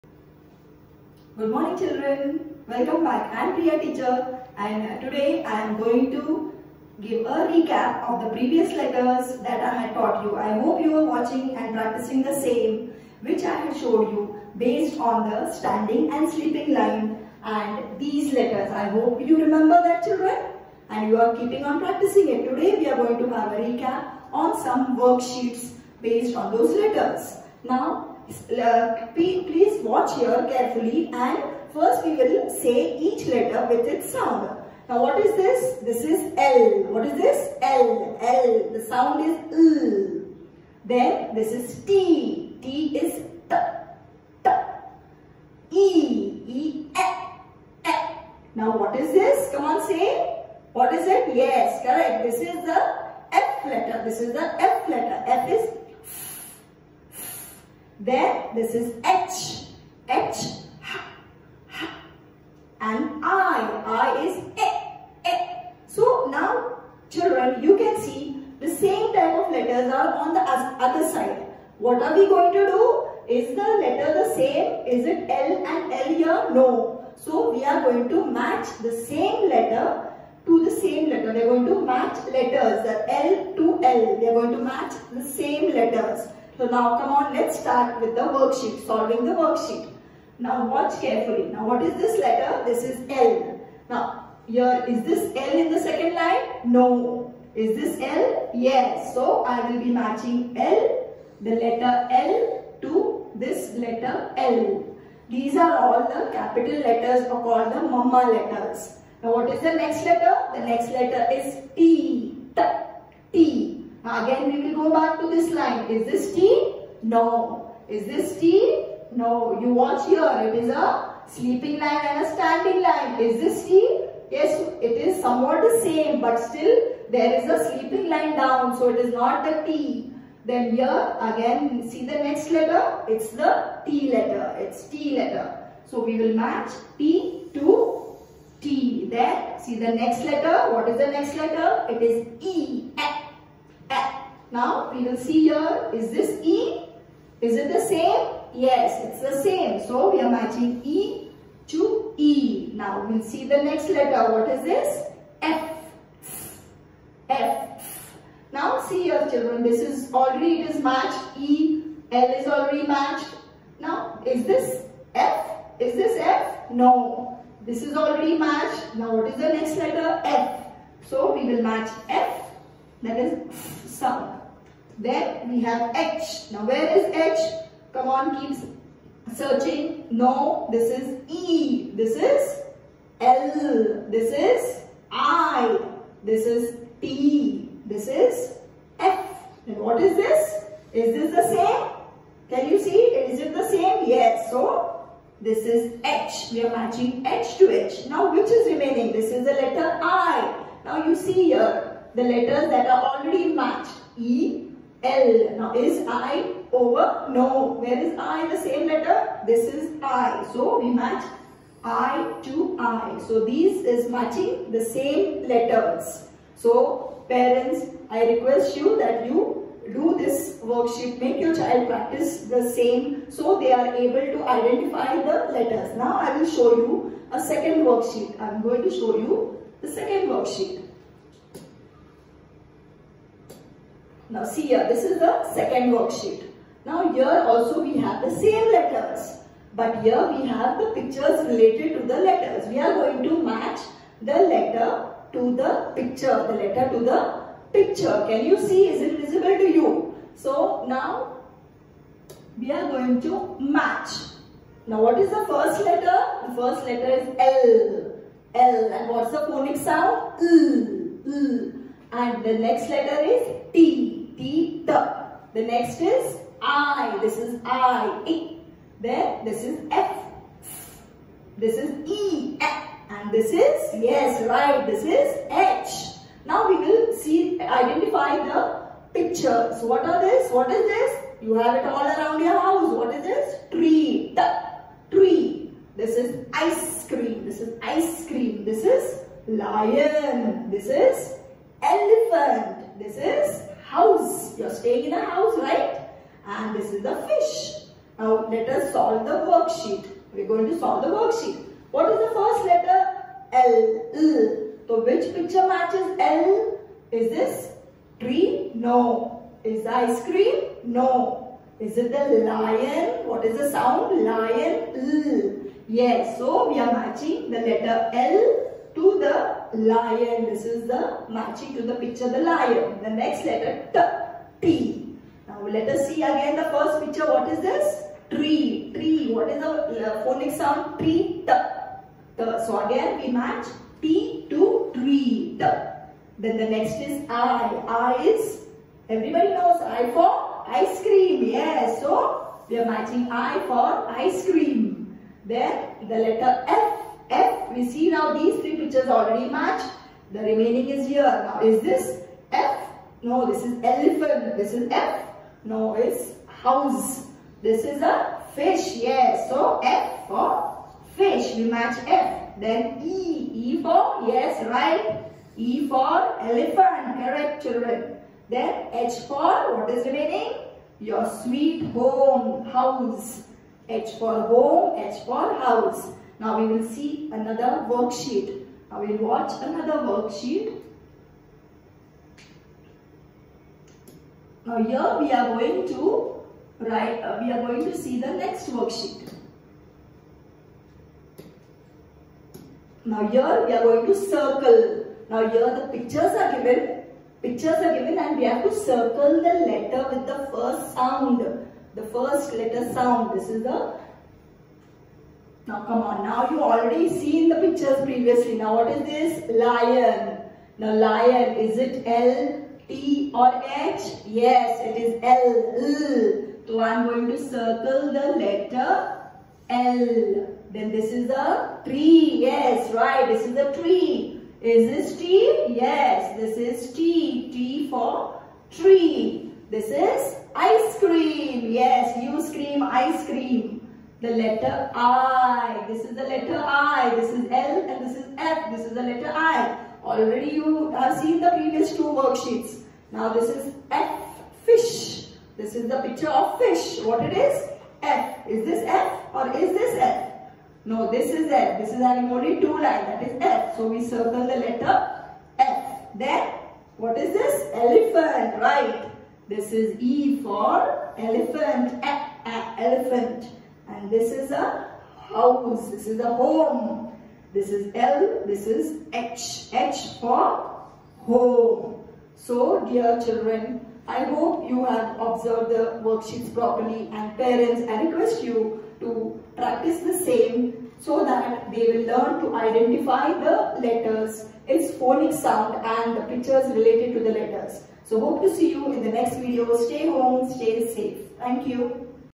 LKG: Revision of Letters
This Video is being prepared by a teacher of St. Mary's Pre-Primary School, Jalna, Maharashtra  to help students Learn at home during  Covid-19 lockdown period. We hope that this small initiative of SMS Jalna will help the students to utilize their time constructively in learning at home when coming to school is restricted. Stay Home Stay Safe.